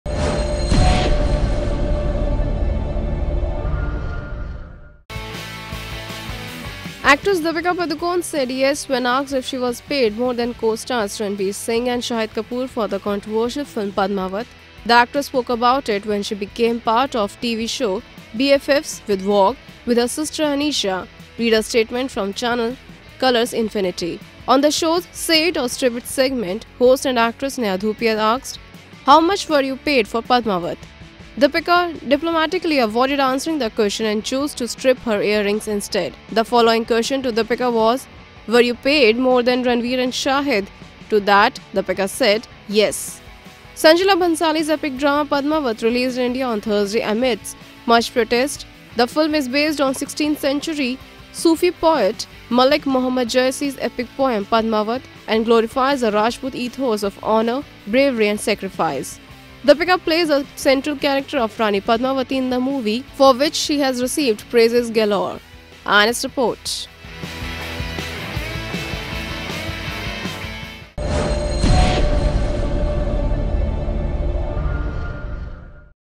Deepika paid more than Ranveer- Shahid | Padmaavat
Sanjay Leela Bhansali's magnum opus film 'Padmaavat' finally made it's way to the theaters and is receiving continuous love and positive response from the celebrities to the public alike. Overwhelmed by the response, Deepika and Shahid at an event said: